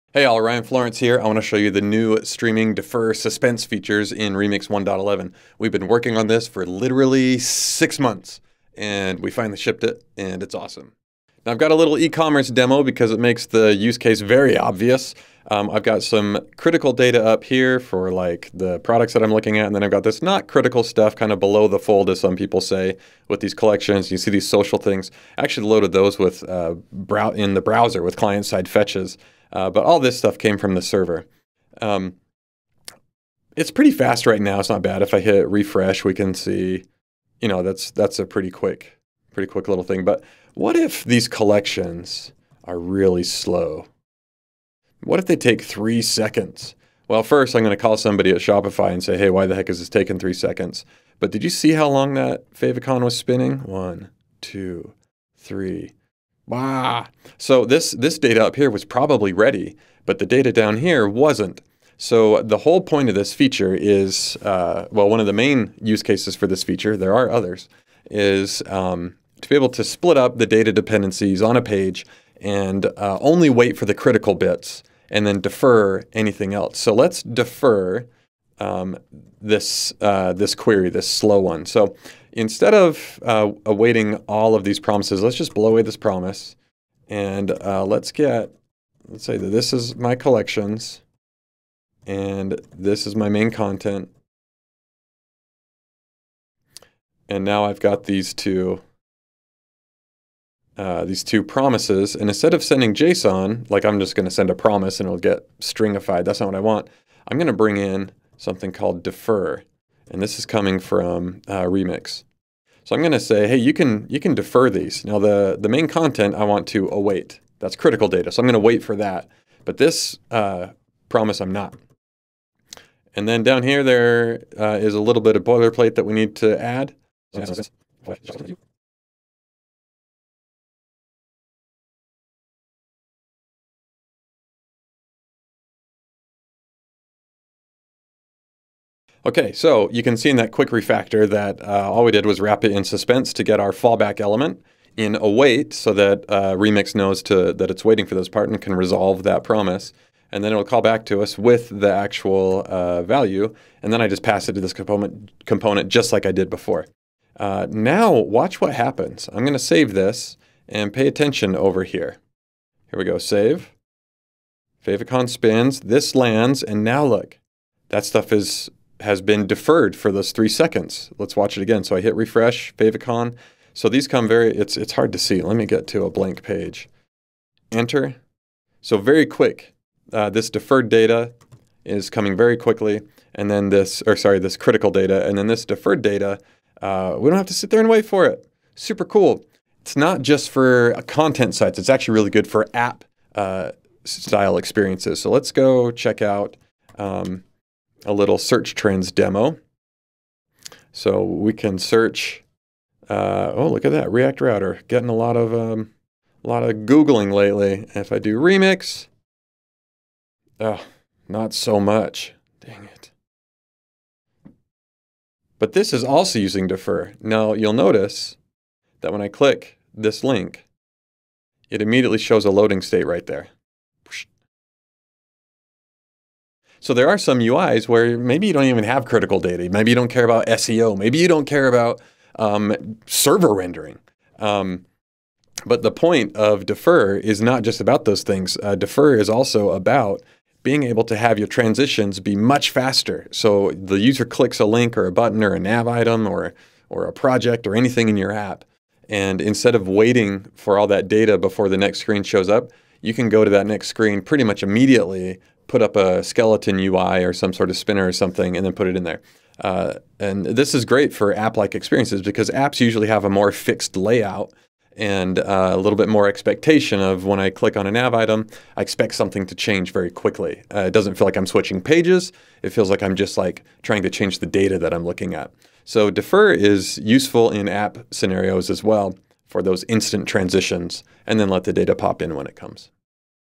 v1.11 Remix Streaming with React Suspense and Remix Defer
Ryan takes us for a first look at Remix Streaming from the v1.11.0 release